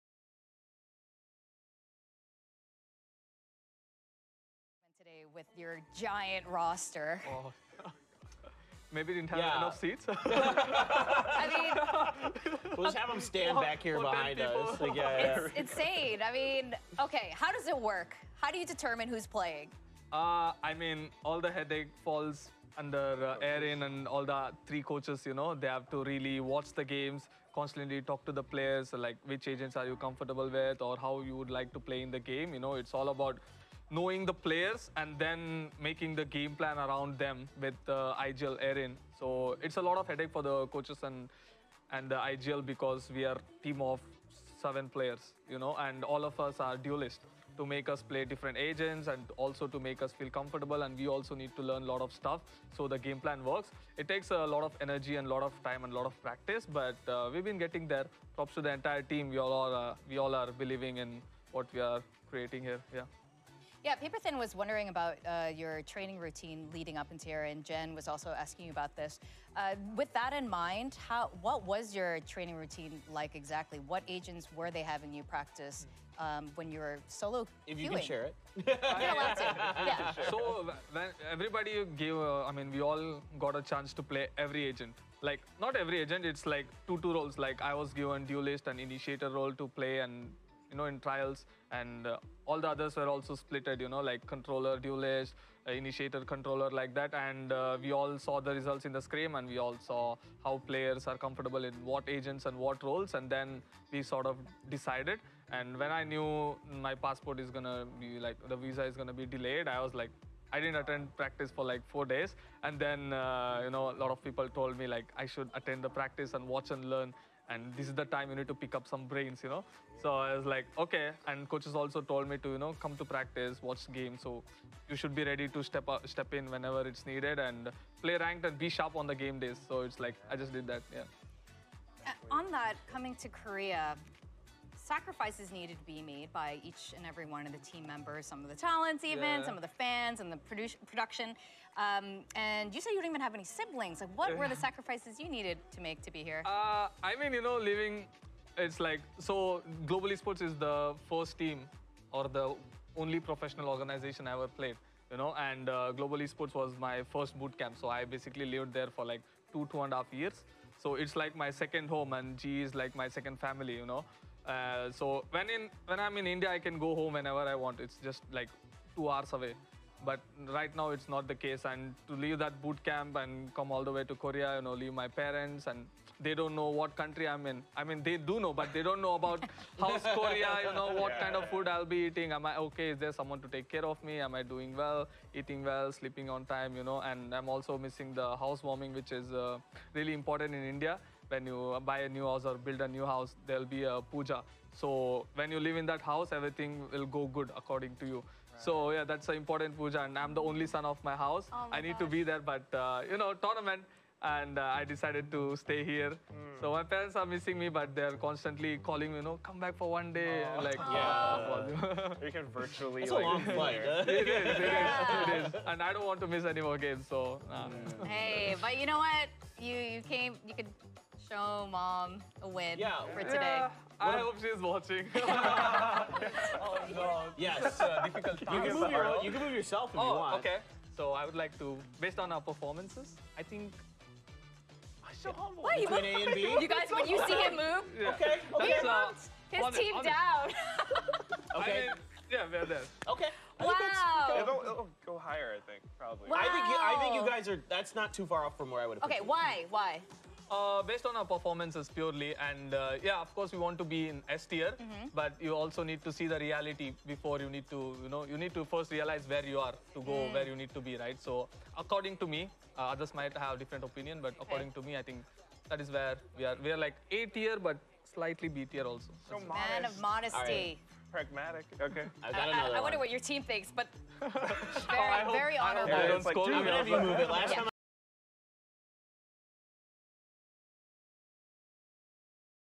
SK Rossi answer some Question at VCT pacific
Valorant Champions Tour League, SK Rossi Answers few questions of analyst's at analyst's Desk and tell how he misses his home